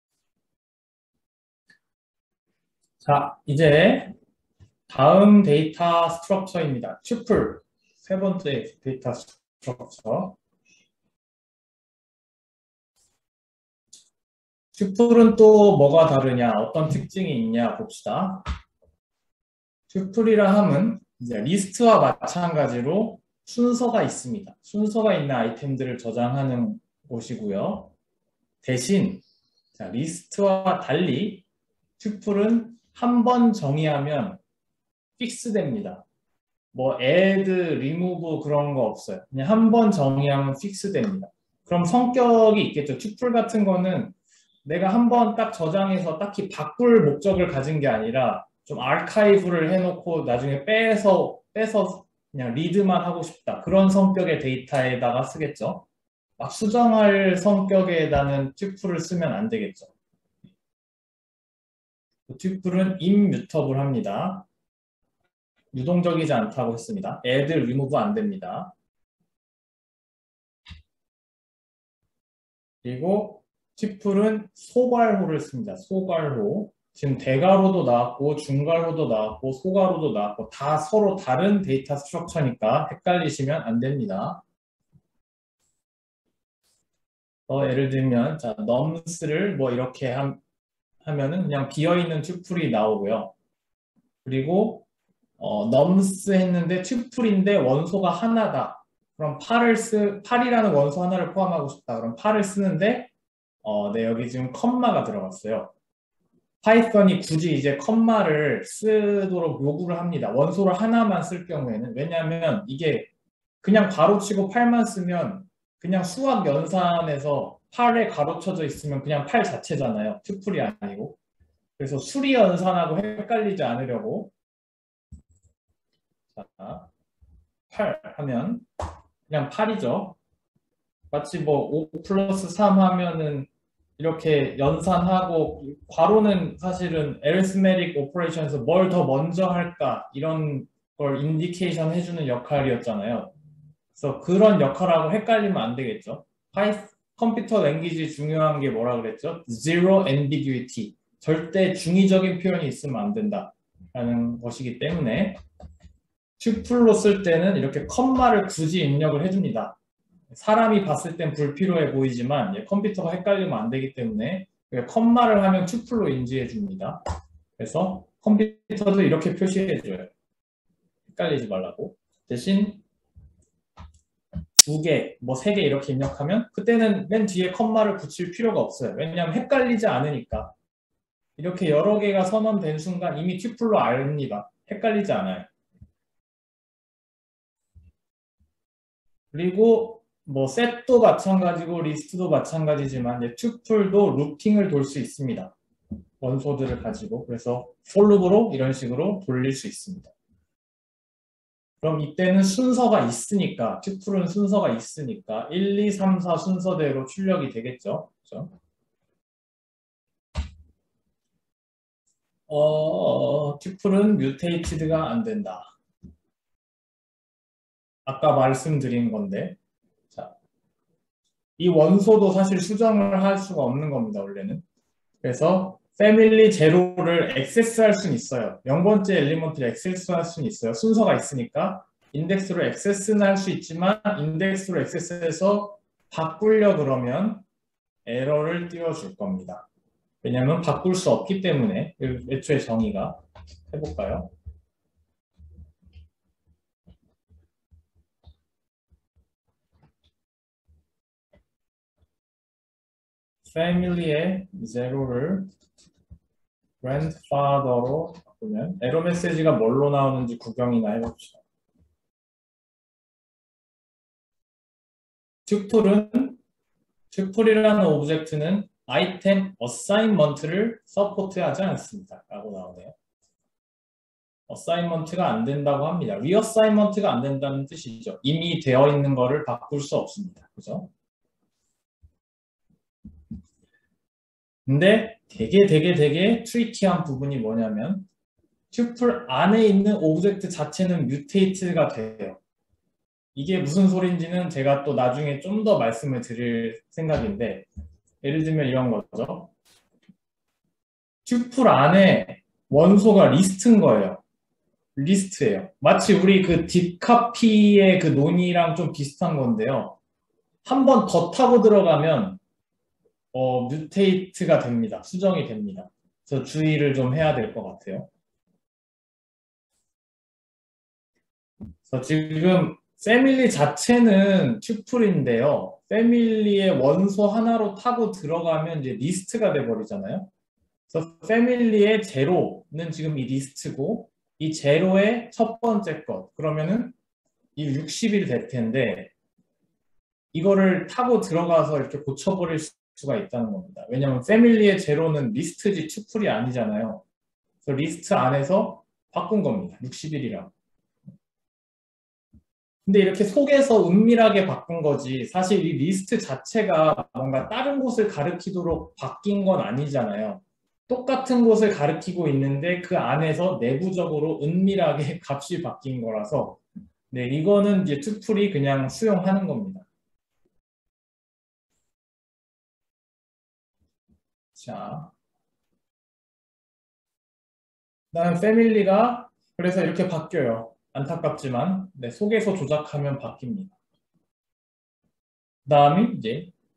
[데사컴기] Lecture 4-2: Tuples
서울대 데이터사이언스 대학원 2021년 2학기 "데이터사이언스를 위한 컴퓨팅의 기초" 수업

비전공자들을 대상으로 Python, 자료구조, 시스템 기초, C 프로그래밍을 다량의 과제를 통해 하드 트레이닝하는 수업입니다 :) 유투브로 실습 관련 영상은 공개하지 않습니다.